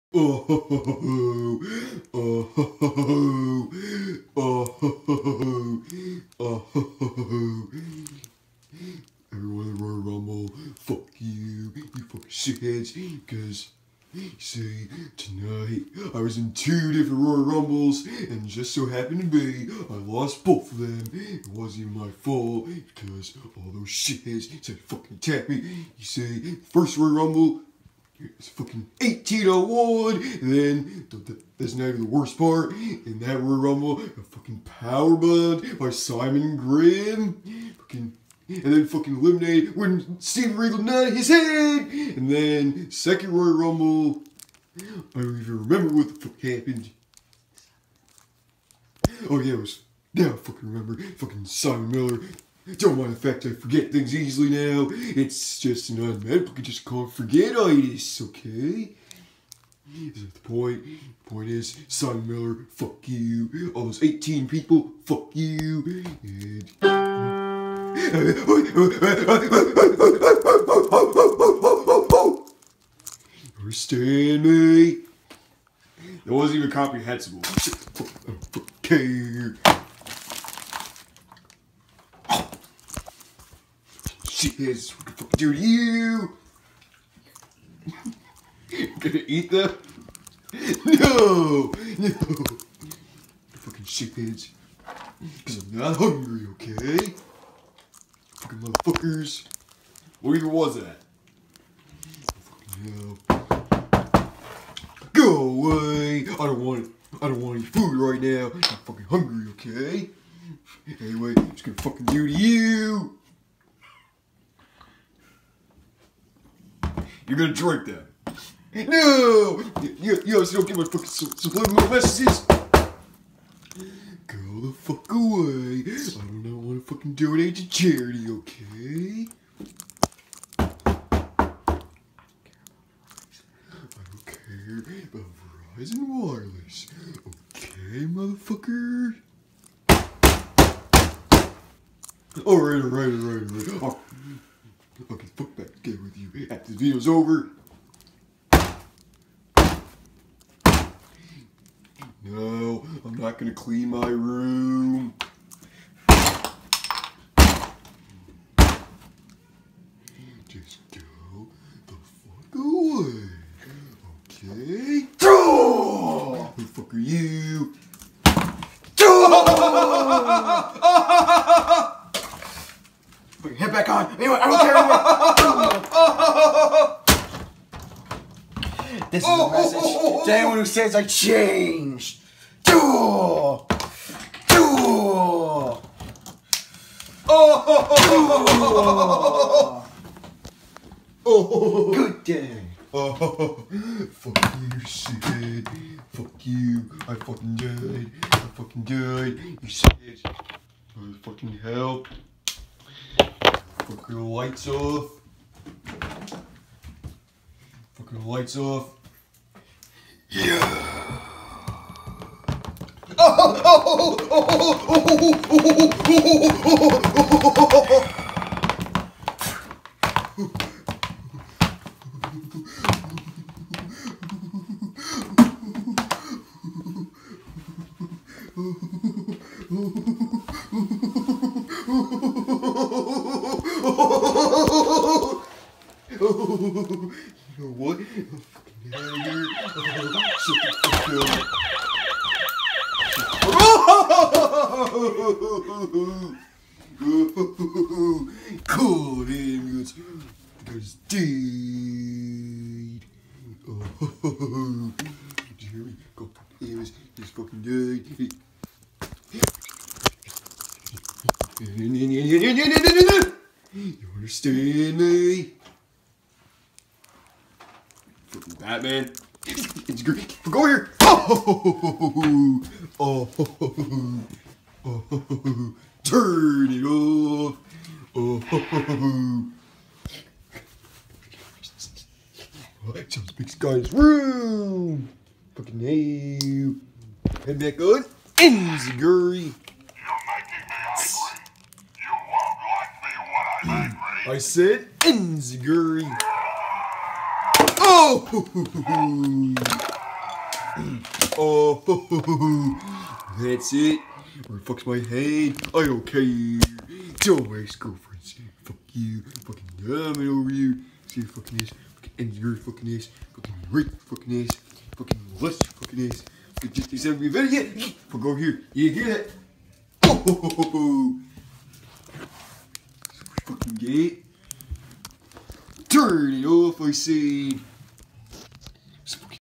IM GONNA FUCK YOU ROYAL RUMBLE
EVERYONE IN THE GREATEST ROYAL RUMBLE RAPED ME, I WAS FUCKING JUMPED BY THOSE 18 PEOPLE, AND MY NUTS POPPED BECAUSE OF IT AND IN THE OTHER ONE I FUCKING DIED AT THE HANDS OF SIMON MILLER, WHY? HERES WHY, BECAUSE I HAVE JUSTIN BIEBER POSTERS IN MY ROOM AND AND YOU SHITHEADS IN THE AUDIENCE NEVER FUCKING HELPED ME, BECAUSE IM RACIST TOWARDS ARABS!!! MARK HENRY NEEDS TO SWITCH BODIES WITH ME, THE SCORPION CAN GO FUCK MY NIPPLES, FUCK MY SHITTY FIRE CAR, IT SHOULDVE SHOVED FIRE UP MY ASS I CHALLENGE THE HULK TO A FUCKING FIGHT, IM GONNA FUCKING GET RAPED AGAIN AND THE CAMERAMAN WILL TOO